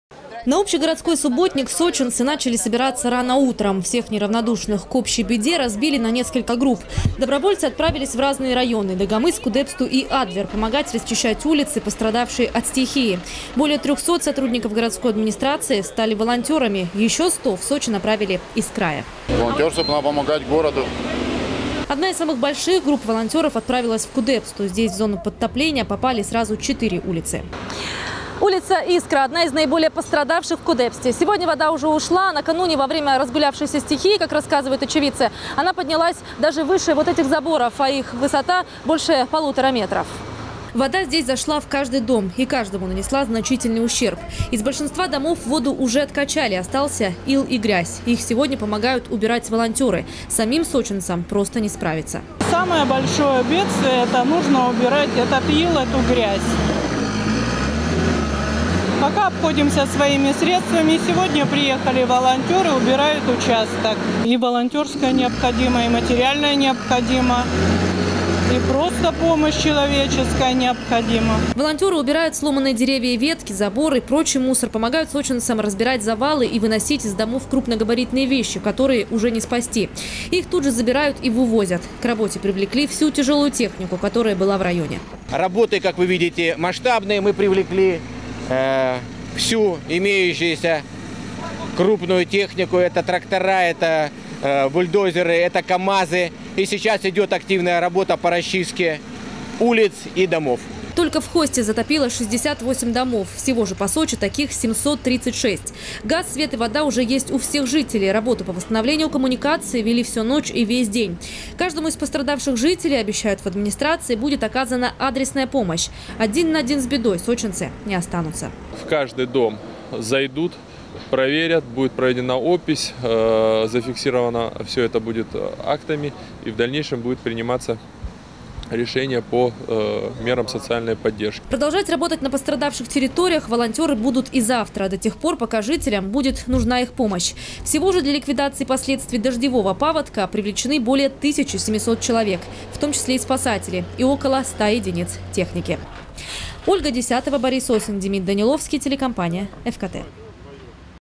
Ликвидировать последствия вчерашней стихии в Сочи помогают волонтёры. Новости Сочи Эфкате
В пострадавших районах они сегодня работали с самого утра. В основном помогали жителям расчищать участки во дворах подтопленных домов. Таких, пострадавших в Сочи более 700.
Сочи. Новости.
Телеканал Эфкате.